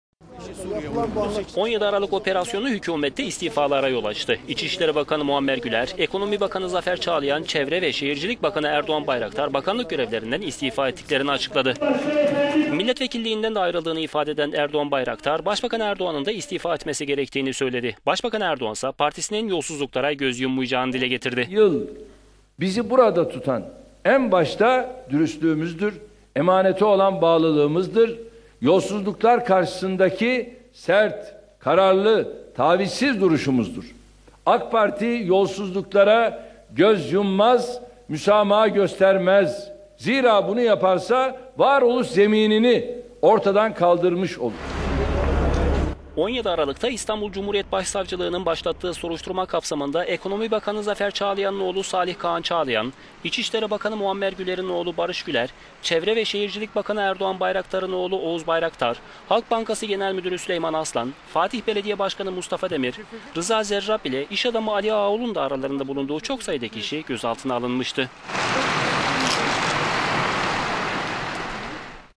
Yolsuzluk operasyonu 4 istifa getirdi
17 Aralık yolsuzluk operasyonu hükumette istifalara yol açtı. İçişleri Bakanı Muammer...

17 Aralık yolsuzluk operasyonu hükumette istifalara yol açtı. İçişleri Bakanı Muammer Güler, Ekonomi Bakanı Zafer Çağlayan ile Çevre ve Şehircilik Bakanı Erdoğan Bayraktar, bakanlık görevlerinden istifa etiklerini açıkladı.

Milletvekilliğinden de ayrıldığını ifade eden Bayraktar, Başbakan Erdoğan'ın da istifa etmesi gerektiğini söyledi.

Öte yandan eski İçişleri Bakanı, AK Parti Milletvekili İdris Naim Şahin partisinden istifa etti.

Başbakan Recep Tayyip Erdoğan ise partisinin yolsuzluklara göz yummayacağını dile getirdi.

Başbakan Erdoğan şöyle konuştu:

"11 yıl bizi burada tutan en başta dürüstlüğümüzdür, emanete olan bağlılığımızdır, yolsuzluklar karşısında sert, kararlı, tavizsiz duruşumuzdur. Ak  Parti yolsuzluklara göz yummaz, müsamaha göstermez, zira bunu yaparsa varoluş zeminini ortadan kaldırmış olur"